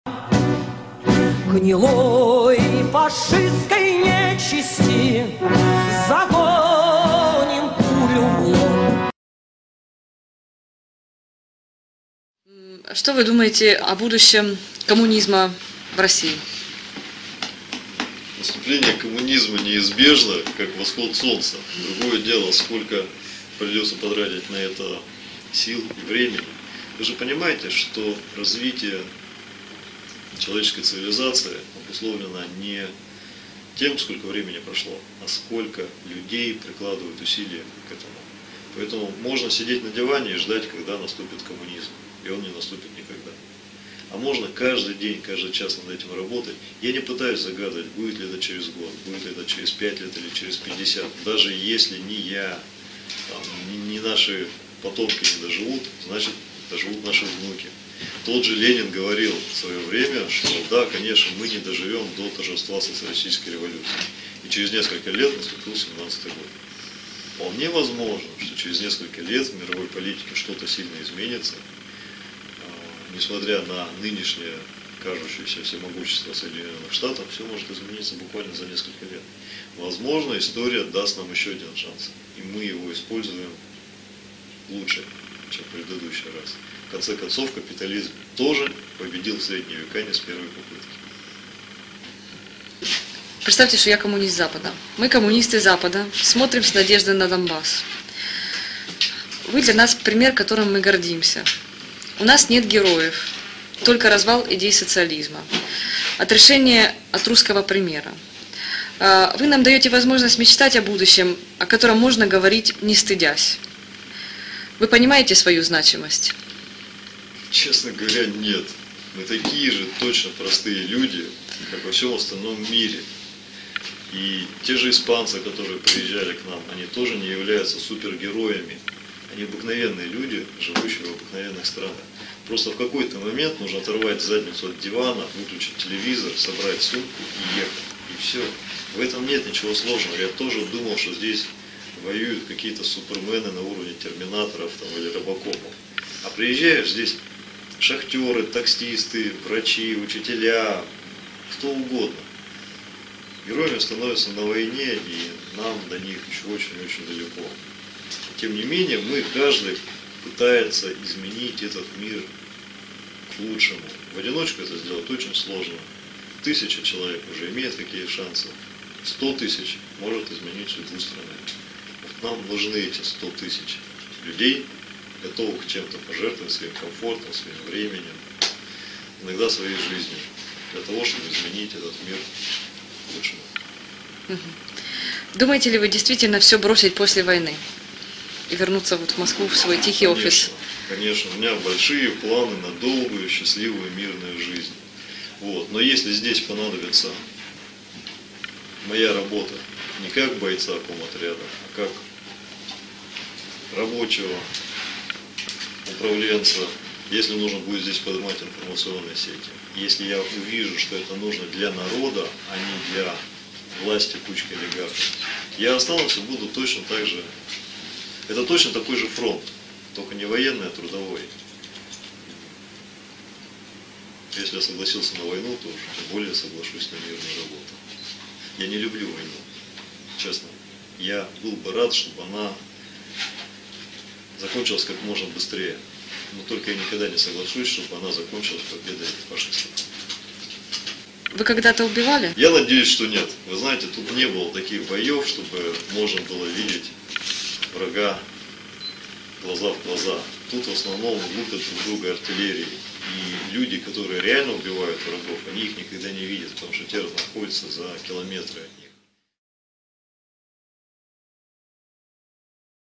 Alexey Markov: Amanecer Rojo (Sub.Castellano)
Entrevista con el comisario político de la unidad 404, Alexey Markov, sobre el futuro del Comunismo.